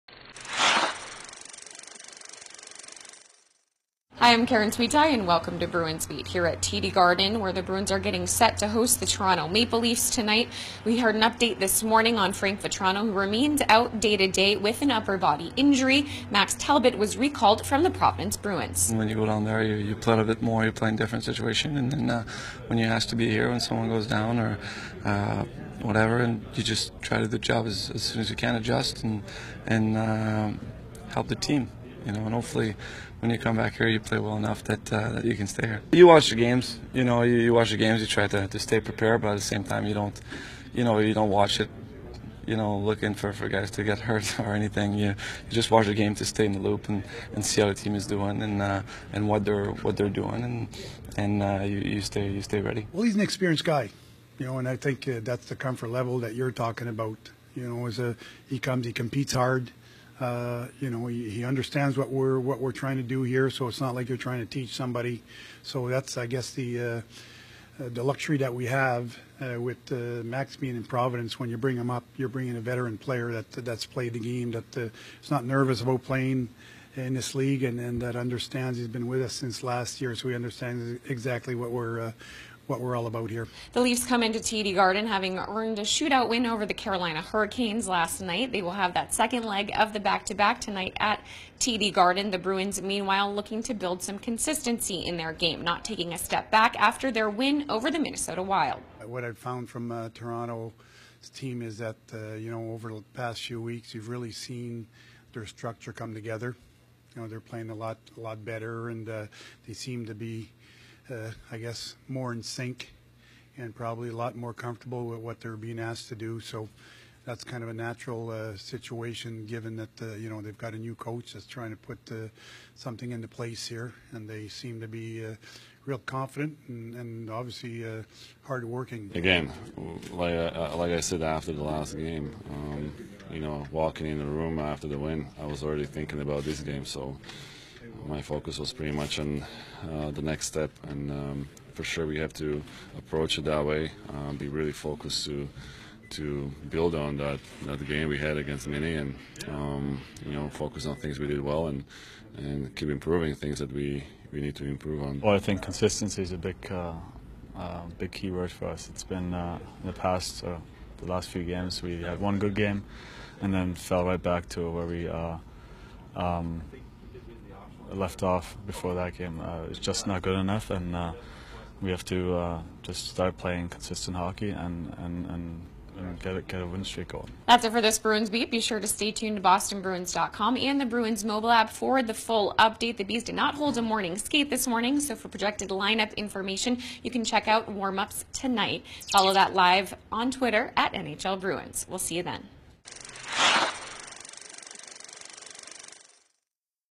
Finalizar com vitória!
Com Max Talbot chamado do P-Bruins, Bruins trabalha firme para fechar com vitória série de 5 jogos seguidos em casa.

Video by Boston Bruins TV